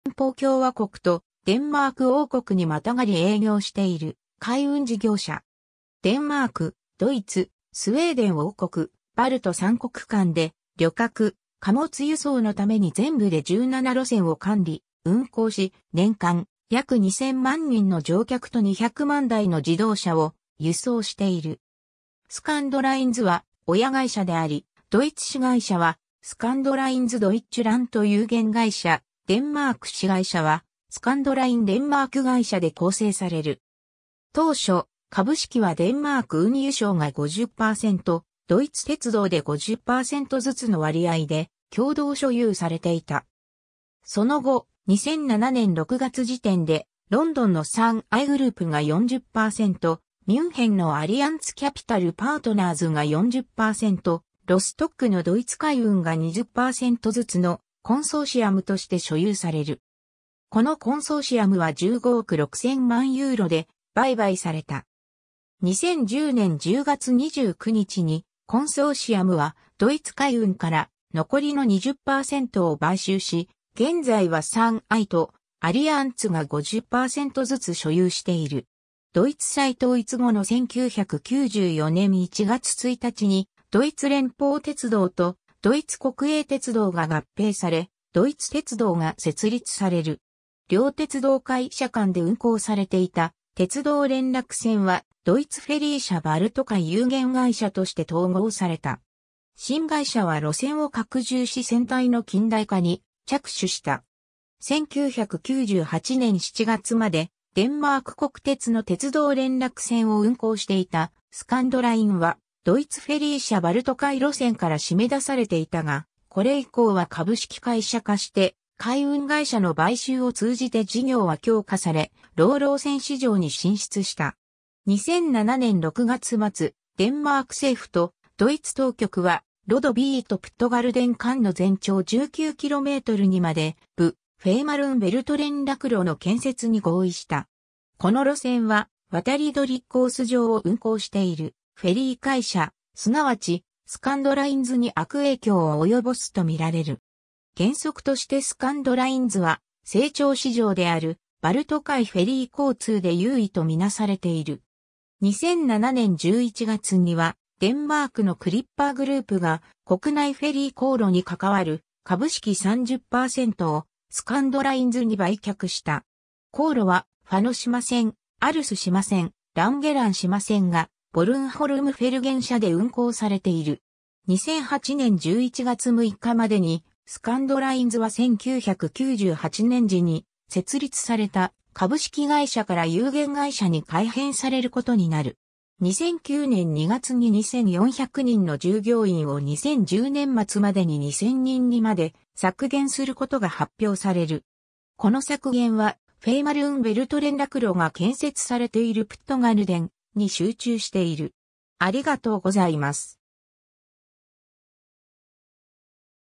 スカンドラインズ
メクレンブルク＝フォアポンメルン州の企業
デンマーク・ドイツ関係
スカンドラインズ（Scandlines）は、ドイツ連邦共和国とデンマーク王国にまたがり営業している海運事業者。
デンマーク、ドイツ、スウェーデン王国、バルト三国間で旅客・貨物輸送のために全部で17路線を管理・運行し、年間約2,000万人の乗客と200万台の自動車を輸送している。
 スカンドラインズは親会社であり、ドイツ子会社はスカンドラインズ・ドイッチュラント有限会社（Scandlines Deutschland GmbH）、デンマーク子会社はスカンドライン・デンマーク会社（die Scandlines Danmark A/S）で構成される。
当初、株式はデンマーク運輸省が50％、ドイツ鉄道で50％ずつの割合で共同所有されていた。
その後、2007年6月時点でロンドンの3iグループ（en:3i）が40％、ミュンヘンのアリアンツ・キャピタル・パートナーズが40％、ロストックのドイツ海運（de:Deutsche Seereederei）が20％ずつのコンソーシアムとして所有される。
このコンソーシアムは15億6000万ユーロで売買された。
2010年10月29日にコンソーシアムはドイツ海運から残りの20％を買収し、現在は3iとアリアンツが50％ずつ所有している。
 ドイツ再統一後の1994年1月1日にドイツ連邦鉄道とドイツ国営鉄道が合併されドイツ鉄道が設立される。
両鉄道会社間で運行されていた鉄道連絡船はドイツ・フェリー社バルト海有限会社（Deutschen Fährgesellschaft Ostsee mbH、DFO）として統合された。
新会社は路線を拡充し船隊の近代化に着手した。
 1998年7月までデンマーク国鉄の鉄道連絡船を運行していたスカンドラインはドイツ・フェリー社バルト海路線から閉めだされていたが、これ以降は株式会社化して海運会社の買収を通じて事業は強化されローロー船市場に進出した。
 2007年6月末、デンマーク政府とドイツ当局はロドビーとプットガルデン間の全長19kmに迄ぶフェーマルン・ベルト連絡路の建設に合意した。
この路線は渡り鳥コース上を運行しているフェリー会社、すなわちスカンドラインズに悪影響をおよぼすと見られる。
原則としてスカンドラインズは成長市場であるバルト海フェリー交通で優位とみなされている。
 2007年11月にはデンマークのクリッパー・グループが国内フェリー航路に関わる株式30％をスカンドラインズに売却した。
航路はファノ島線（Fanøtrafikken）、アルス島線（Alstrafikken）、ランゲラン島線（Langelandstrafikken）がボルンホルムフェルゲン社（de:BornholmerFærgen）で運行されている。
 2008年11月6日までにスカンドラインズは1998年時に設立された株式会社から有限会社に改編される事になる。
 2009年2月に2,400人の従業員を2010年末までに2,000人にまで削減することが発表される。
この削減はフェーマルン・ベルト連絡路が建設されているプットガルデンに集中している。